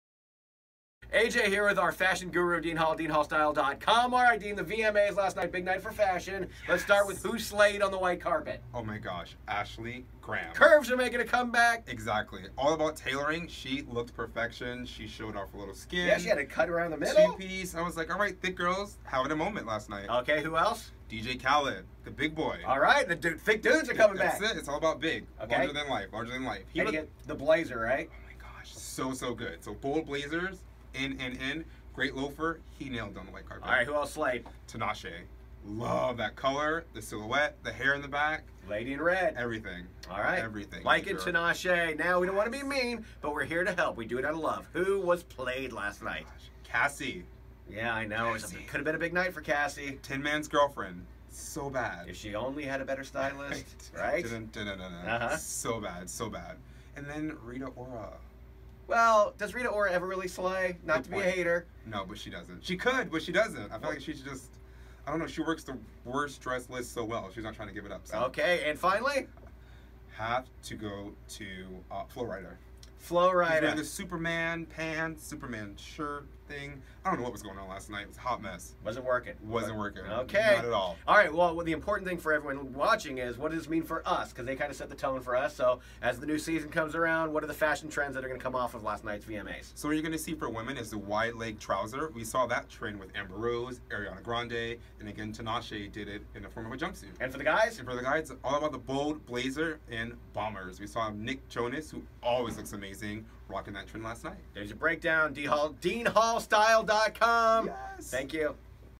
VMAs Fashion with AJ and Stylist Dean Hall
Dean Hall breaks down the good and the bad in fashion for the 2016 VMA's!